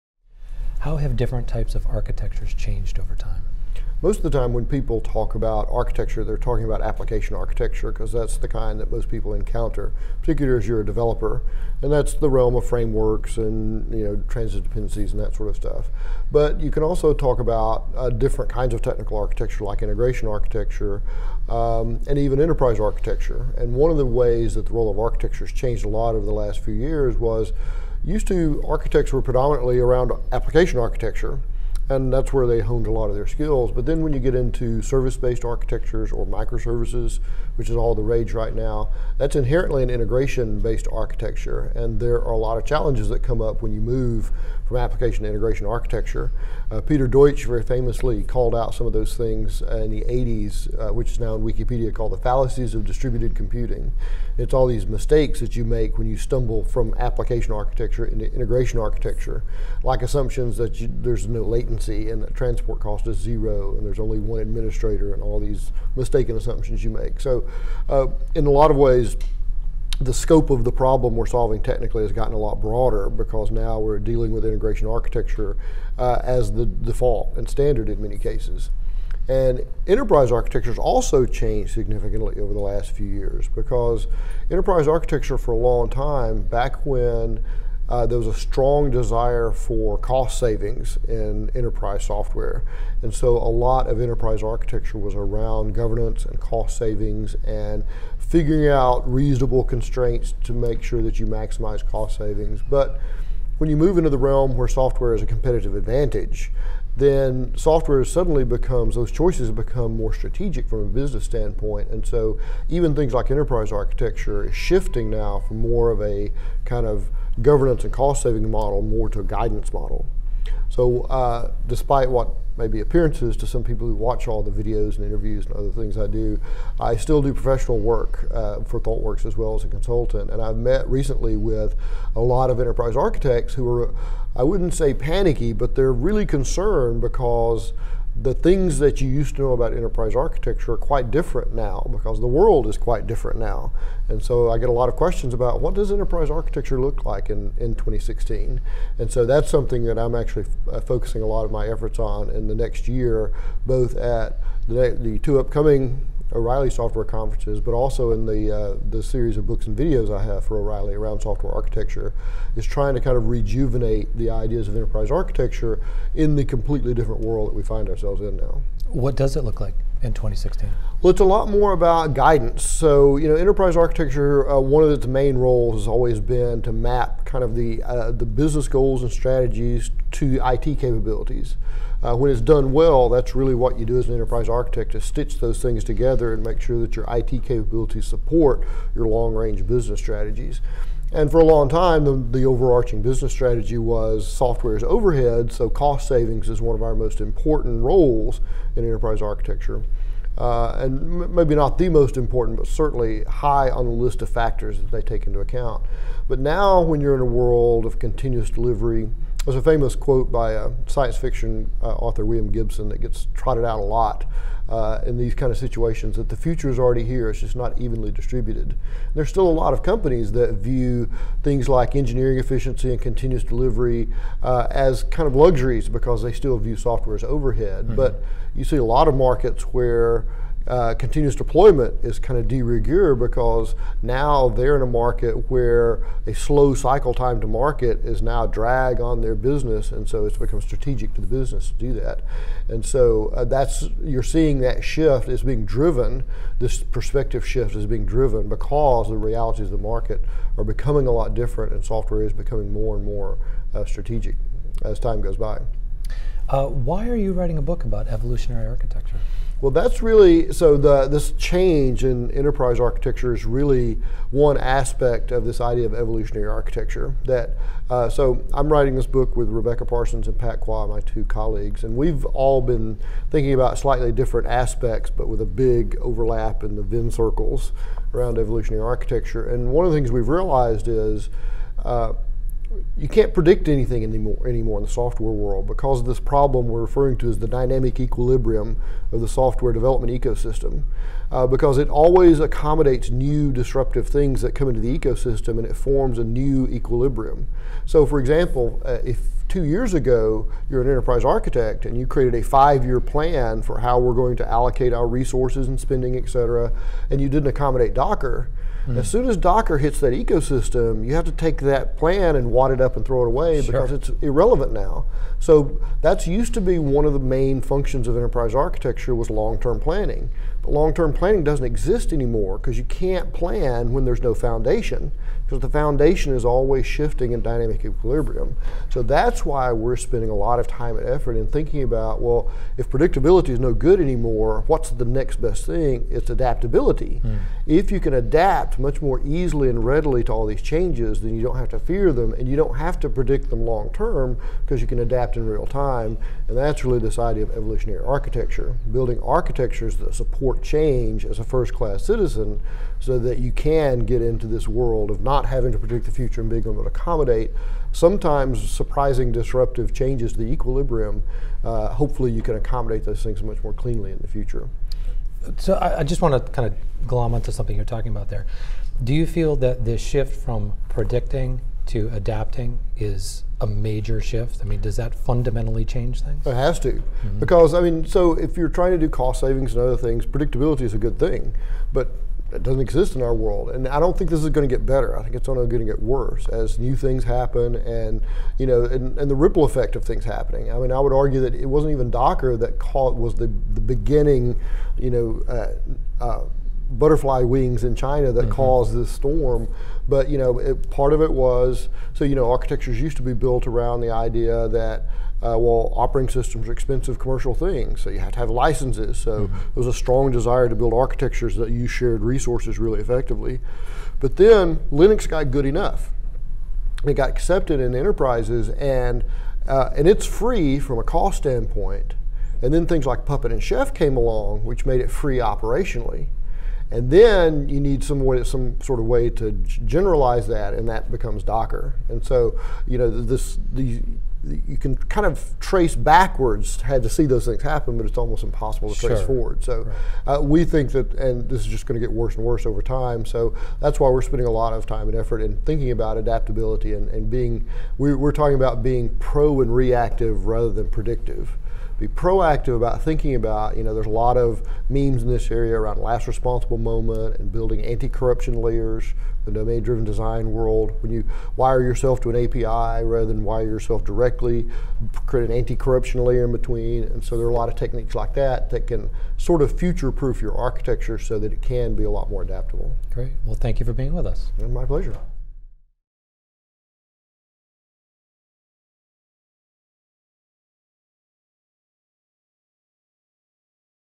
How architectures are changing - Interview with Neal Ford
Neal Ford, Software Architect at ThoughtWorks, explains how different types of architecture have changed over time. Ford also shares his thoughts on what enterprise architecture looks like in 2016, as well as reviewing the shift from predicting to adapting.

Neal Ford interviewed by Mac Slocum at Software Architecture NY 2016.